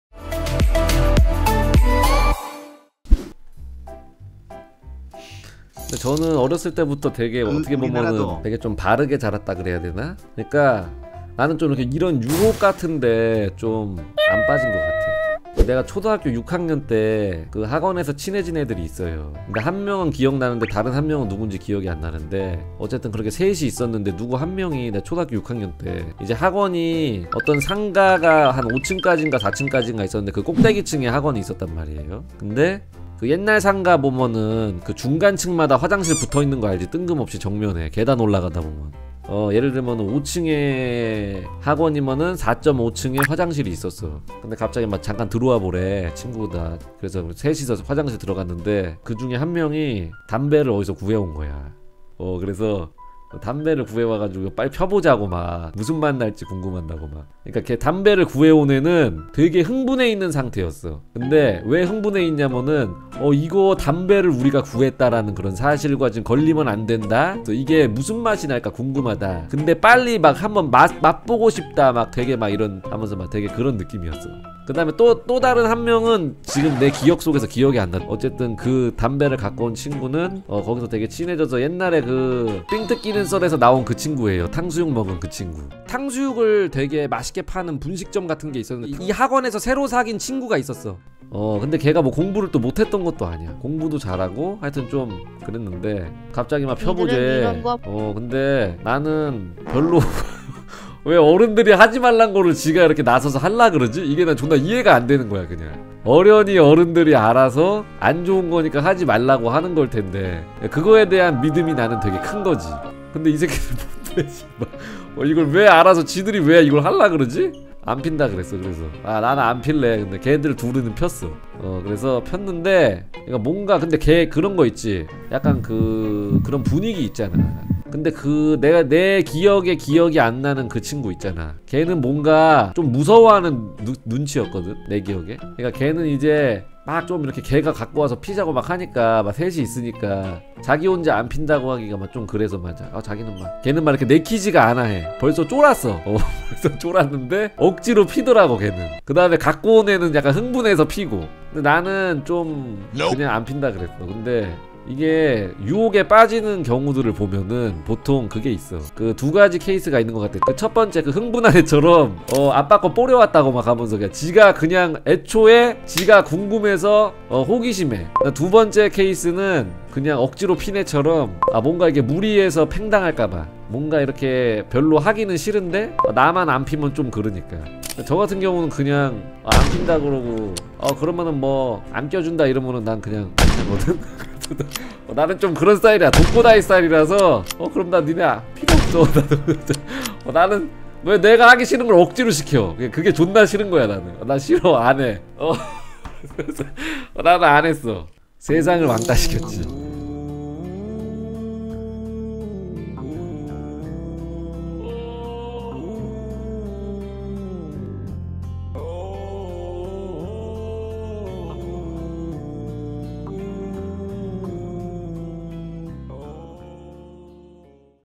초등학교 때 친구 2명이와서 담배를 권한 썰 - 왁굳의 노가리
● 우왁굳 카카오톡 아이디      : @우왁굳
● 영상 업로드 시간                  :  매일 오전 중
 트위치 게임 스트리머 우왁굳의 유튜브입니다 :)